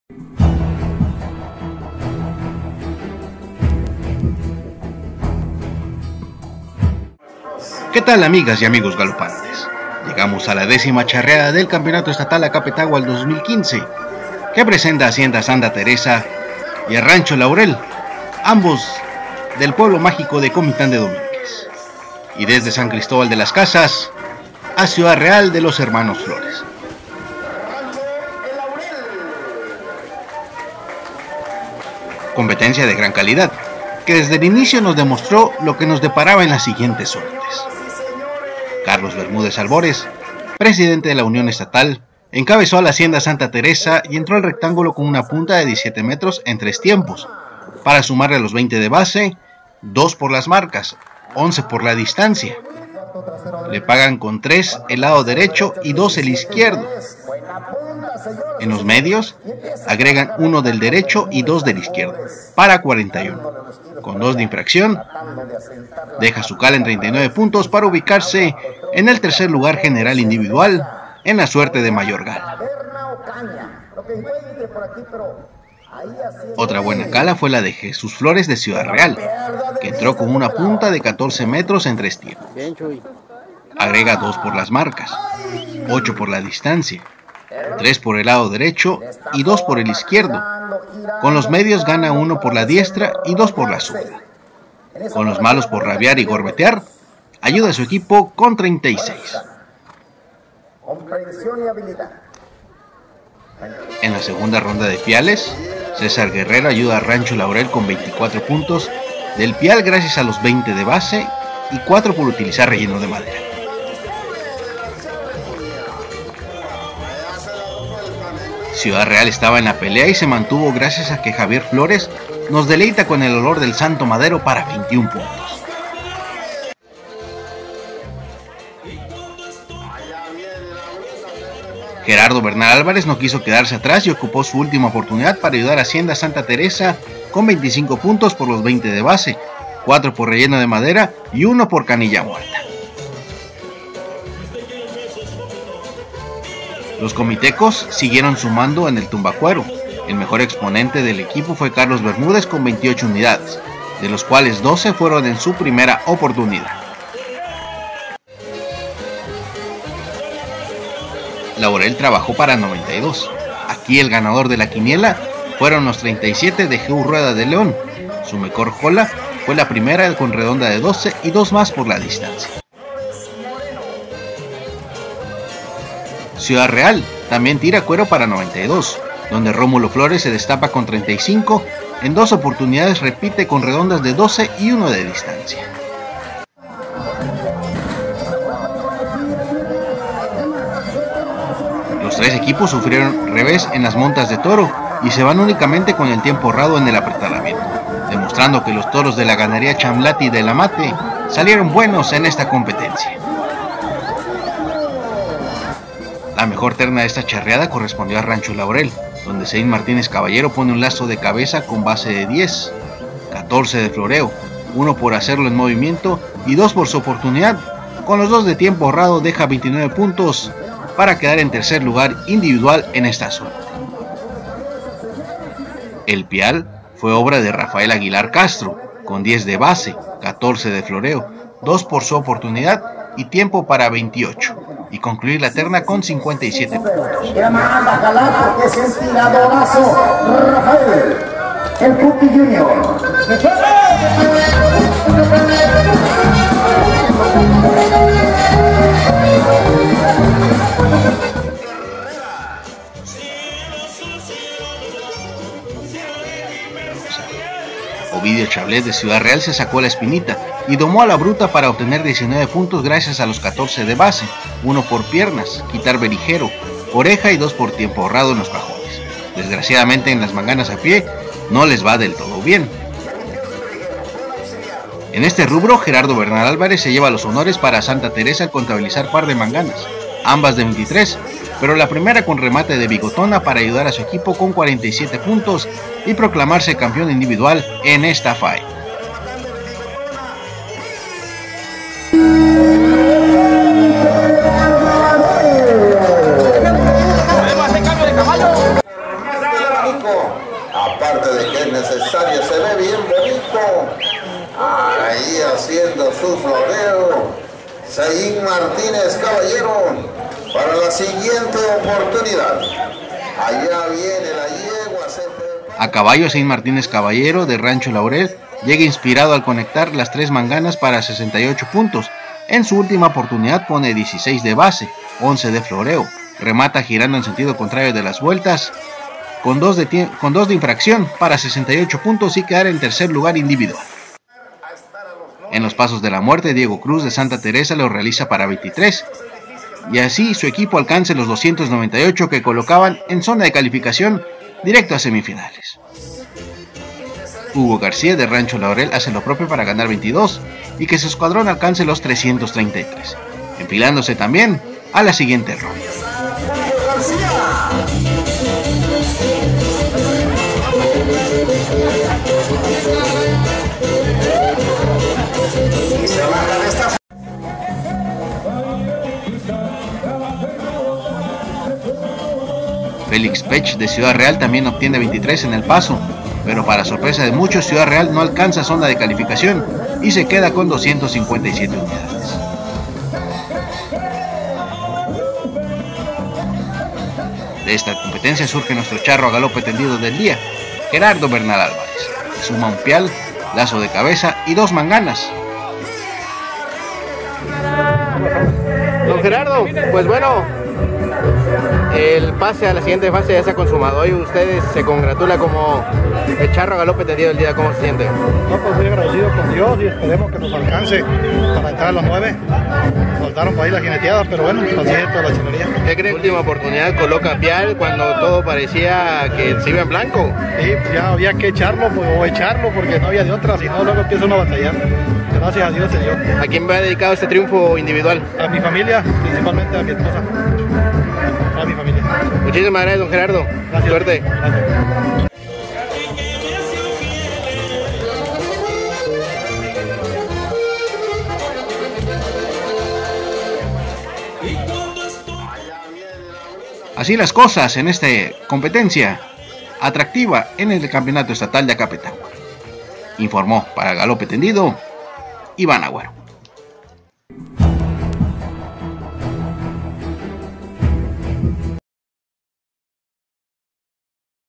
INFORME A GALOPE TENDIDO 15.11
Informe A Galope Tendido del L Campeonato Estatal Charro Acapetahua 2015. En esta competencia intervinieron Hacienda Santa Teresa y Rancho El Laurel de Comitán, así como Ciudad Real de San Cristóbal de Las Casas.